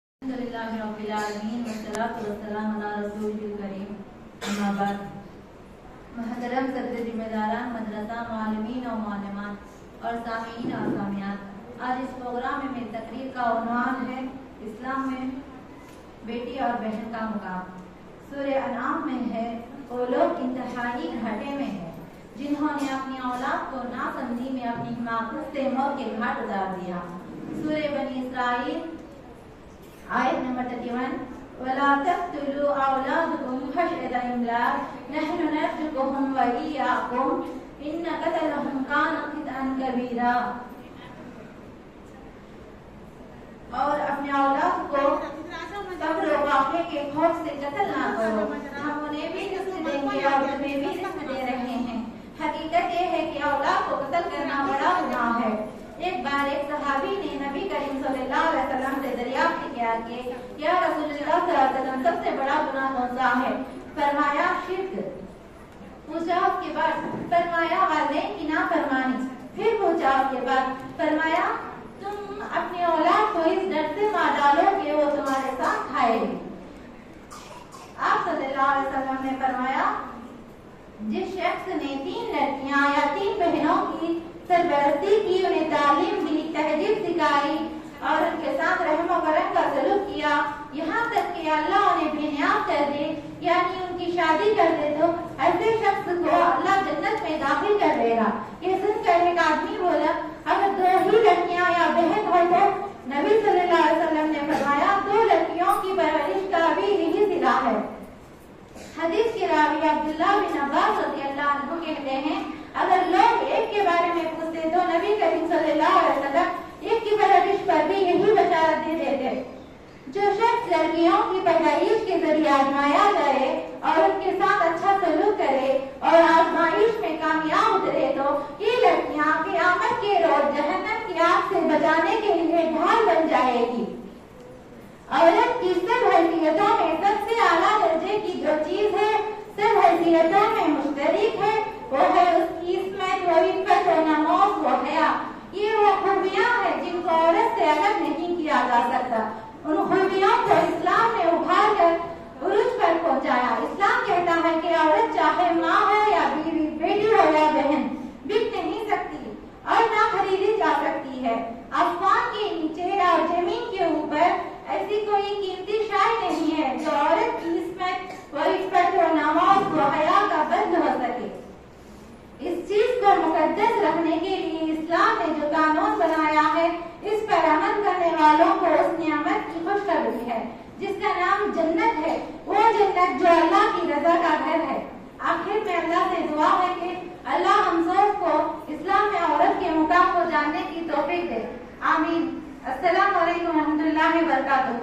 Islam Me Bety Aur Bahen Ki Ahmiyyat Talibah Madrasa Markazut Tauheed lilbanat
Markaz Masjid E Tauheed Ahle Hadees Mukundar Gadda Rajampet

Seerat E Sahabiyyat
Aqeedah Classes
Hifz E Quraan Aur Hadees
Qasasul anbiya